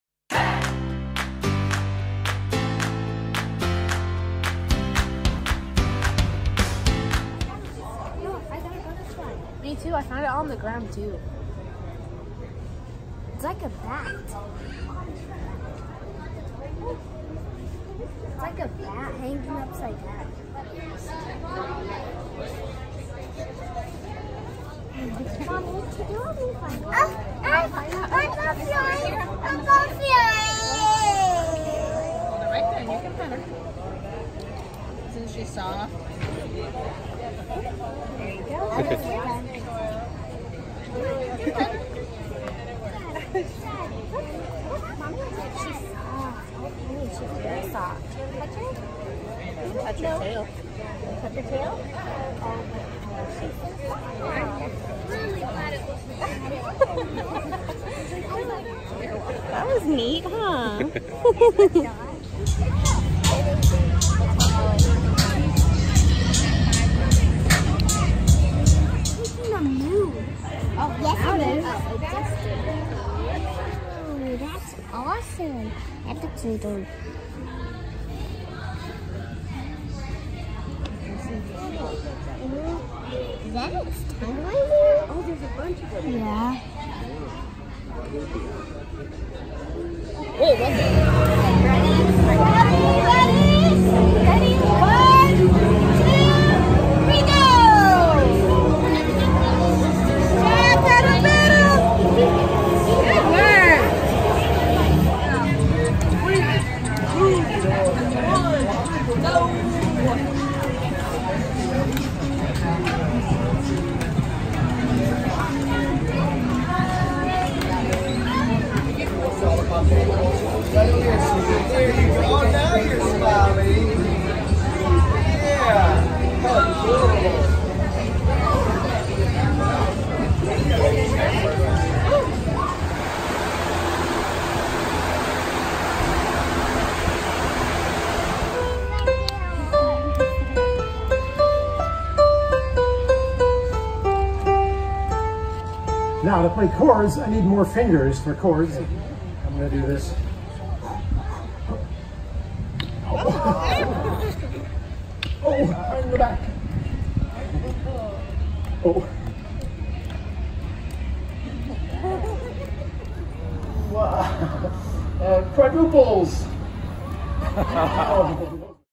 We visited the Jackson County Fair!
We took a trip to the Jackson County Fair, which was much different than the one we would go to in Texas, but we still had fun!